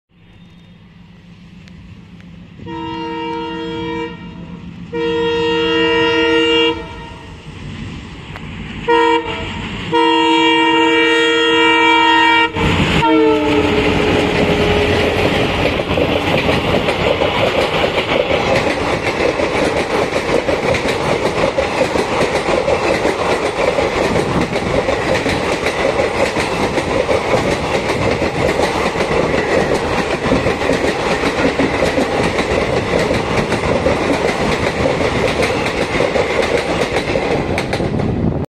Late Running Train | 17651 Chengalpattu - Kacheguda Express with GTL ALCO Twins | TheRailgram
Extremely Late Running 17651 Chengalpattu - Kacheguda Express Skipping Mahbubnagar Town Halt.
Late Running Due to the continuous and heavy rains in Chennai - Renigunta section. The Chengalpattu-Kacheguda Express was diverted via Katpadi, Pakala, Dharmavaram and Gooty instead of Tiruttani and Renigunta.

GTL WDM3D  #11437 + WDM3A  #16658

D.O.C 23 November 2021 12:25 pm